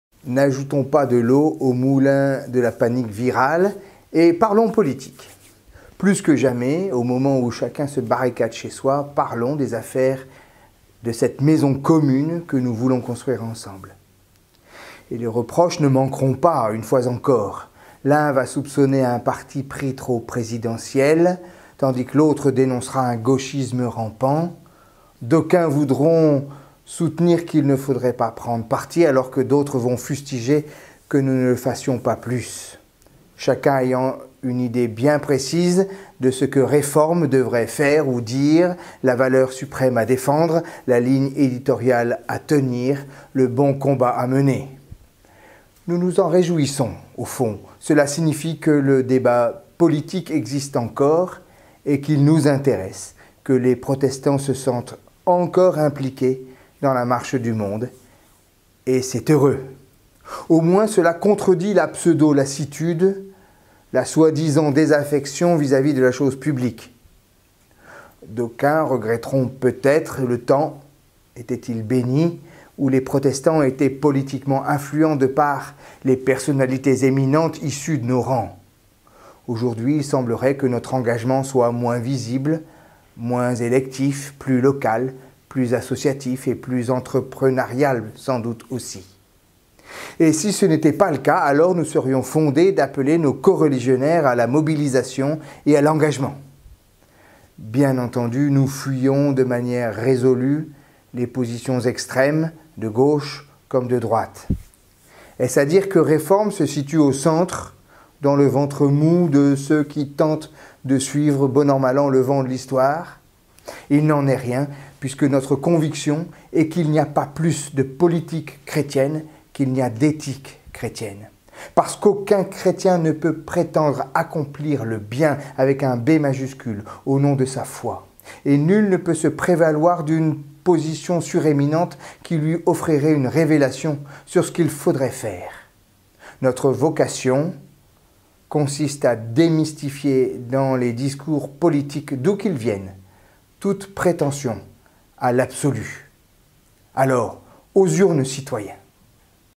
Sur la corde raide – l'édito du pasteur Samuel Amédro
Sur la corde raide  – l'édito du pasteur Samuel Amédro, conseiller théologique de Réforme.

www. reforme.net